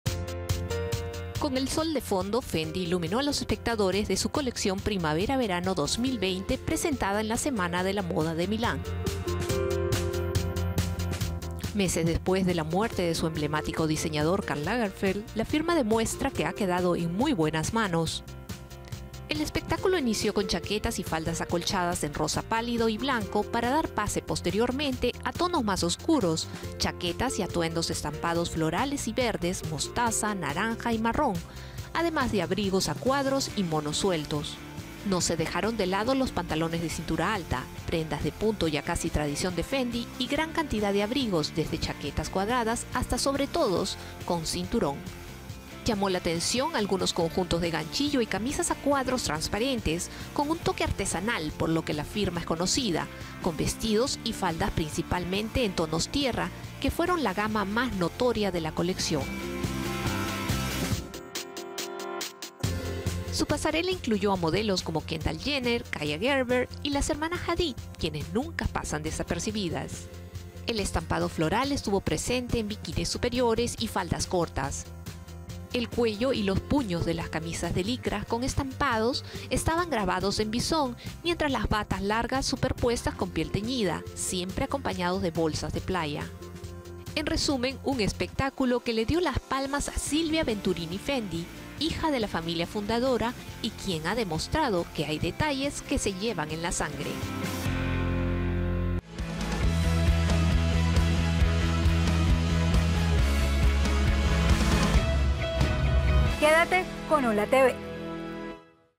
Las icónicas modelos que formaron parte del desfile de Fendi en Milán Fashion Week 2019 | ¡HOLA! TV
Fendi deslumbró a los espectadores con su colección Primavera- verano 2020 en la Milán Fashion Week 2019 y su propuesta incluyó conjuntos de ganchillo y camisas transparentes a cuadros. Kendall Jenner, Kaia Gerber y las hermanas Bella y Gigi Hadid formaron parte del desfile.